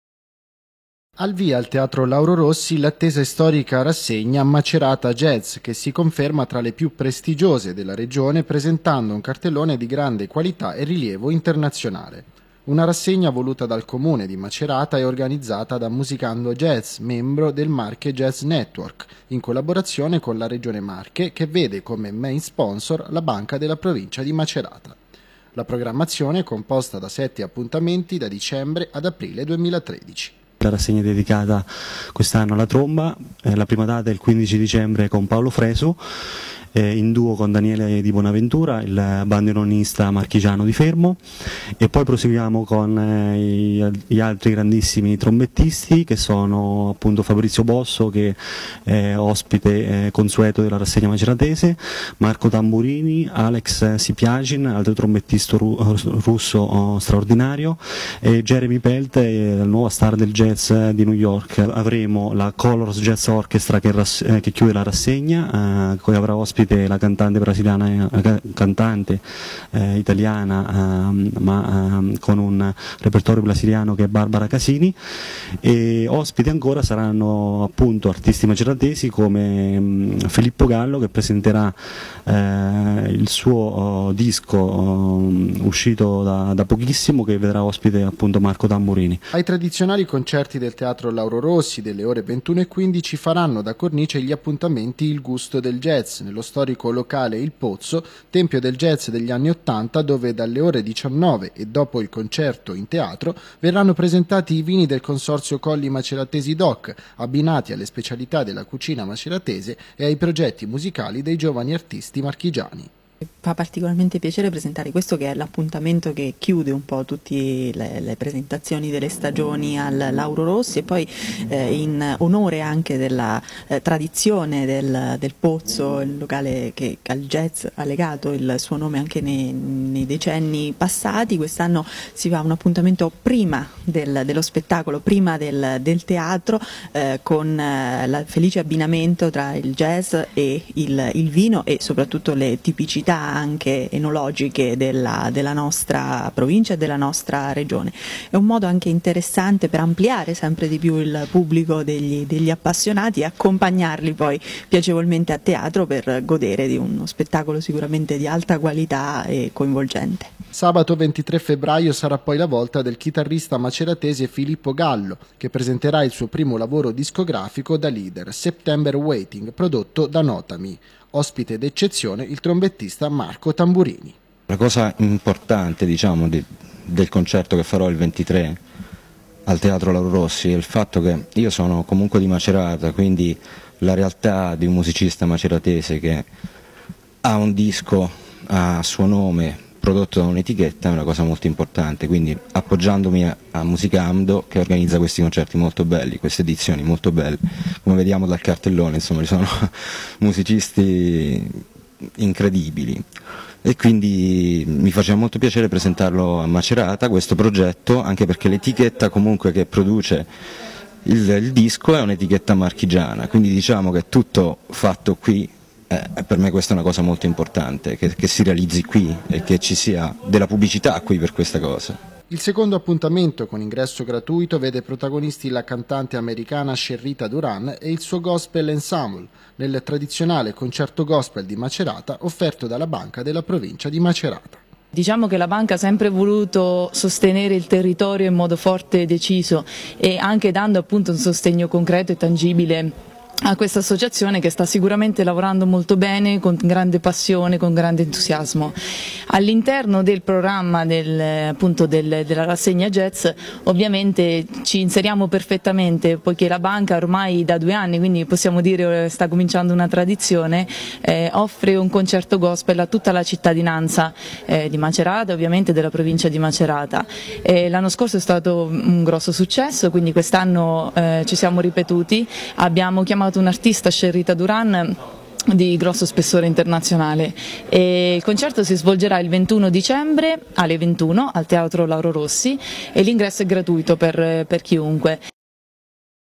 diario LA RASSEGNA JAZZ AL LAURO ROSSI
Diario Maceratese 391. Presentati i sette appuntamenti della Rassegna Jazz 2012/2013 del Teatro Lauro Rossi di Macerata. Tante le eccellenze in cartellone. Servizio di Claudio Ricci.